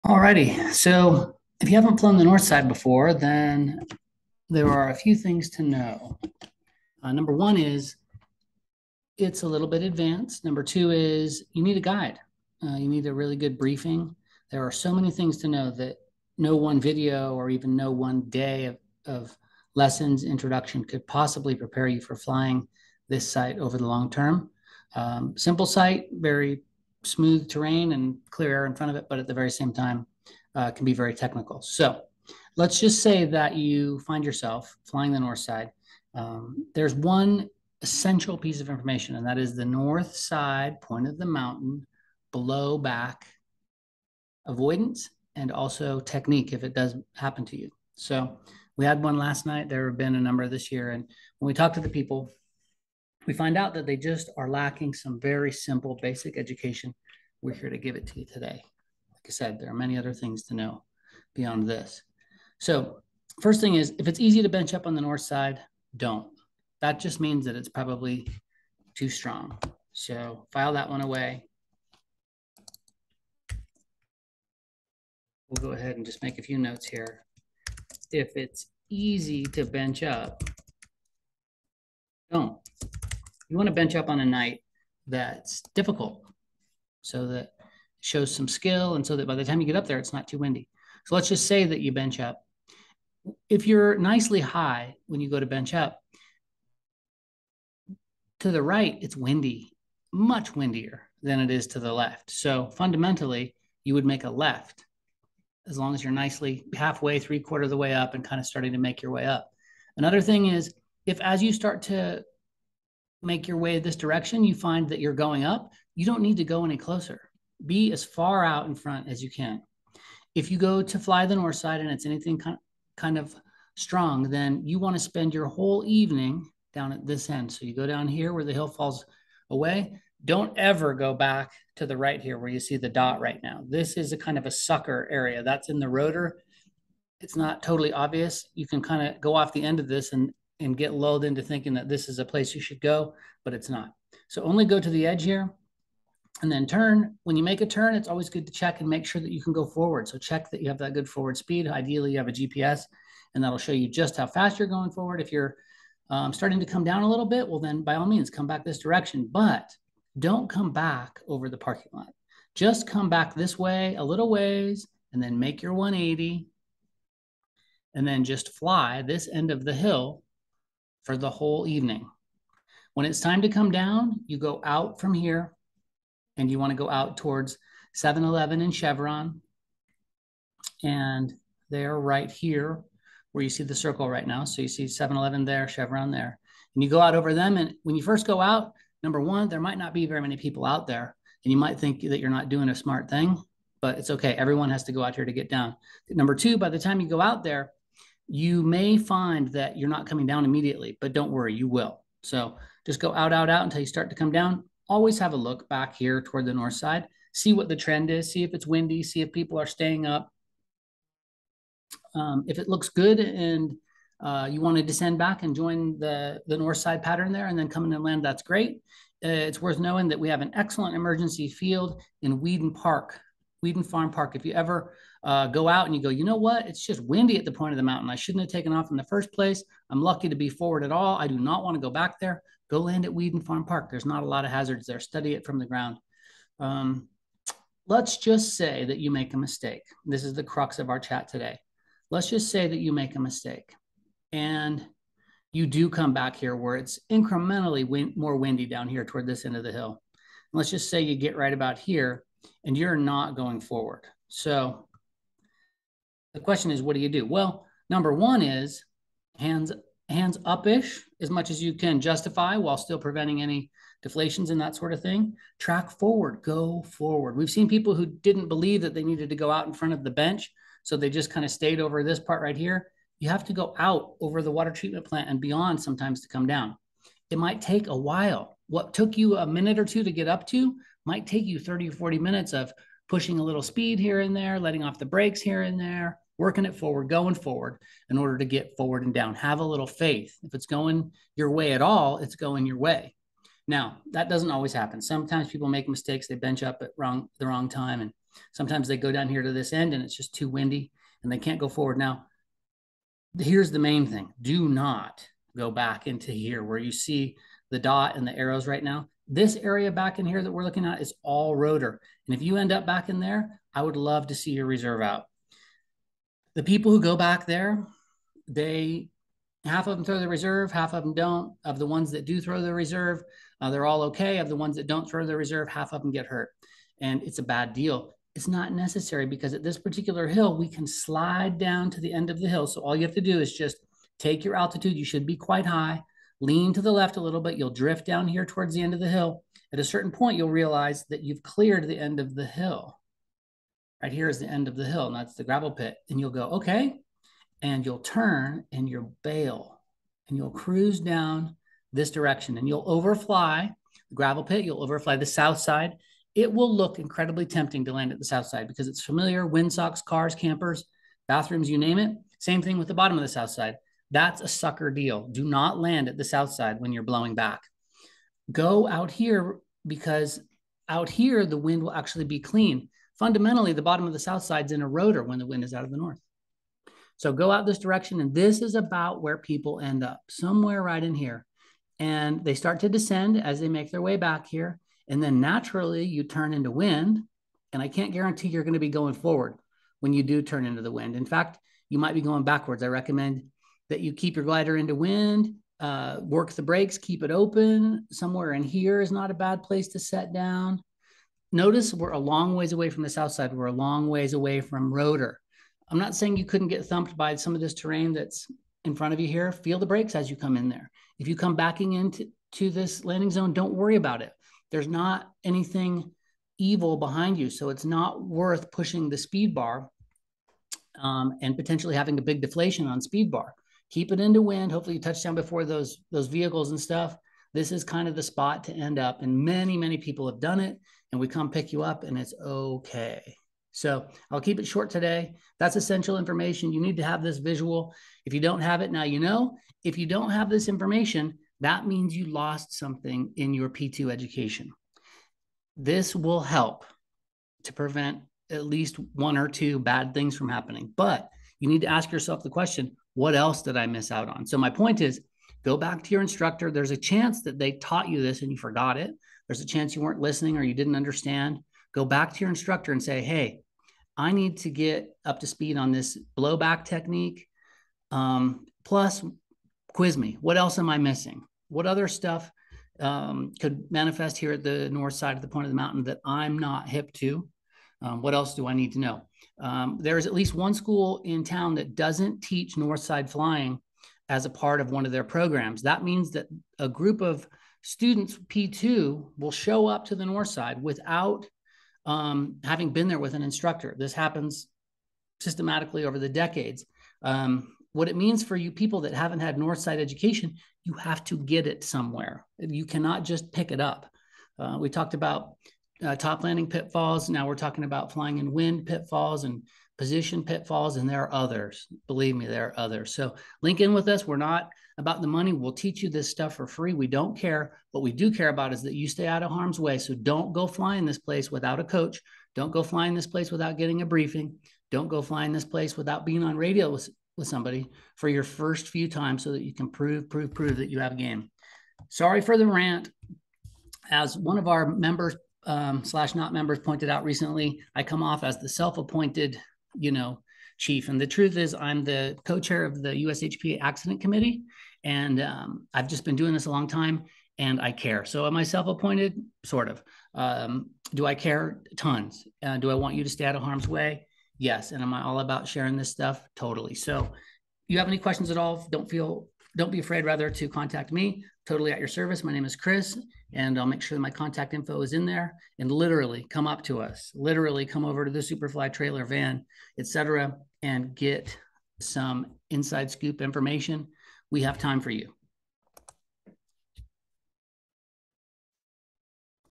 North Side Point of the Mountain Blow Back Avoidance and Procedure
SEE NOTES BELOW ( in the description )

Tiny bit of under education about flying the North Side, here is one of the more important bits of information about flying this site. Please check this info with your instructor to make sure that they agree.

N. Side blowback avoidance and procedure
If its easy to bench up - don’t

If you are high when you bench up, make a left ( save making a right for when your are still fighting for altitude )

Fly the East end of the hill, you don’t come over the parking lot high almost ever
Check your forward speed

If it gets windy, go out front (Wheadon Farm Park)

If its strong, work it forward let off sometimes when you can, push gas if the air is nice enough, it might take a while - don’t do ears until out in front

Don’t blow back behind north side

If blowing back, take your height and slide to the end then turn southbound toward Adobe Building, overfly the SS (don’t land there)

Turn into wind, don’t push gas unless you are at risk of blowing into an object, keep it into wind, keep it open

PLF - check in via FB, telegram etc. have a buddy